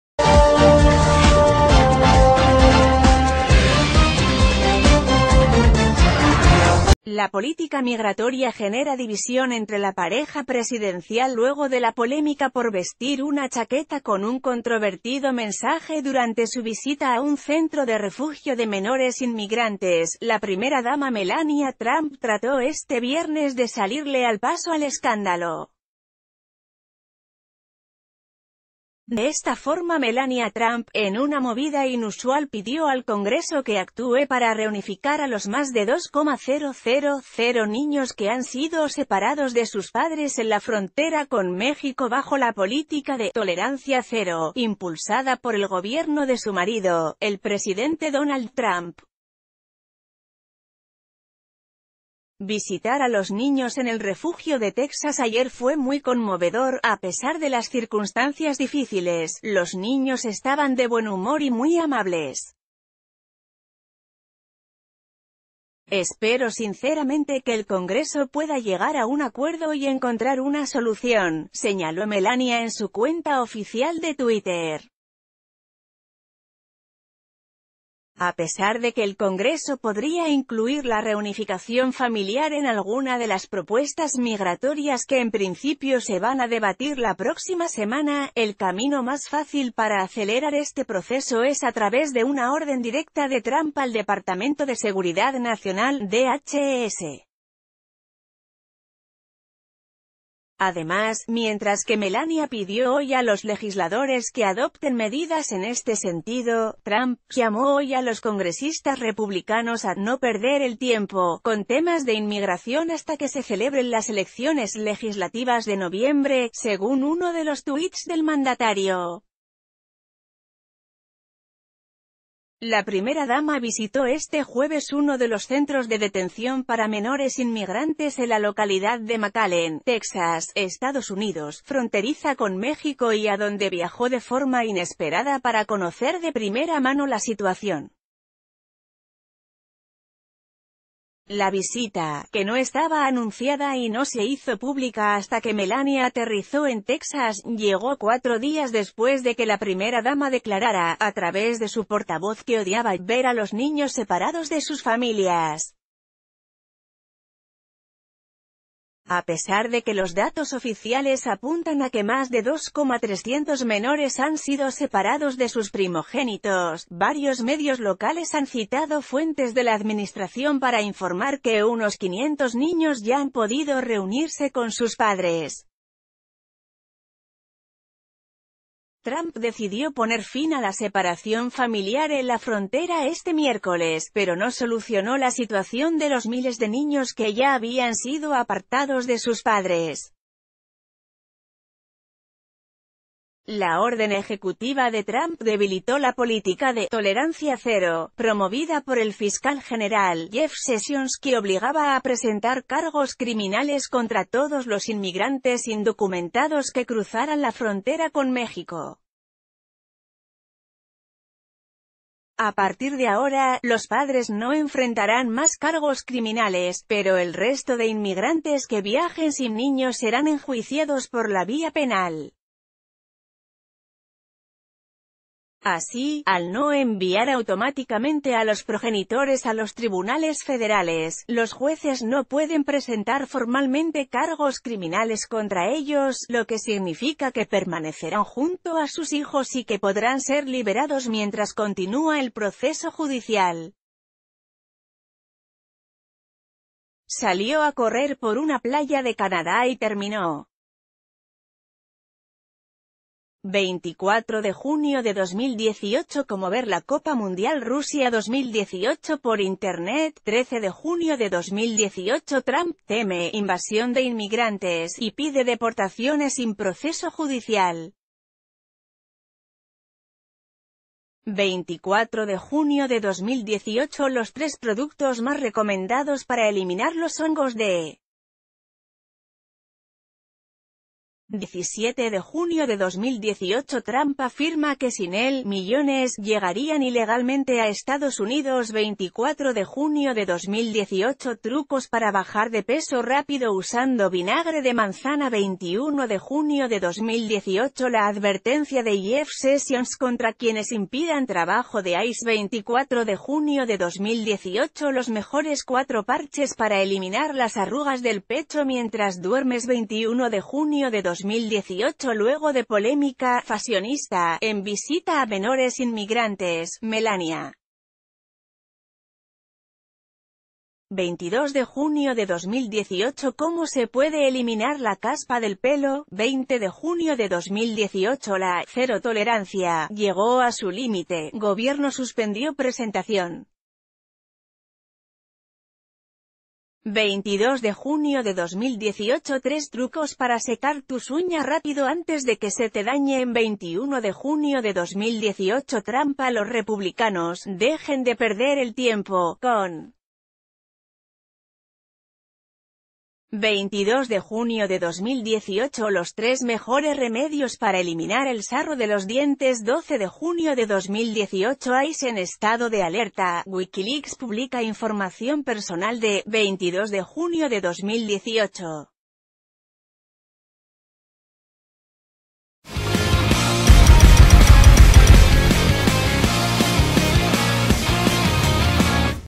Luego de polémica “fashionista” en visita a menores inmigrantes, Melania contradice abiertamente ...
Luego de polémica “fashionista” en visita a menores inmigrantes, Melania contradice abiertamente a Trump

La política migratoria genera división entre la pareja presidencial Luego de la polémica por vestir una chaqueta con un controvertido mensaje durante su visita a un centro de refugio de menores inmigrantes, la primera dama Melania Trump trató este viernes de salirle al paso al escándalo. De esta for...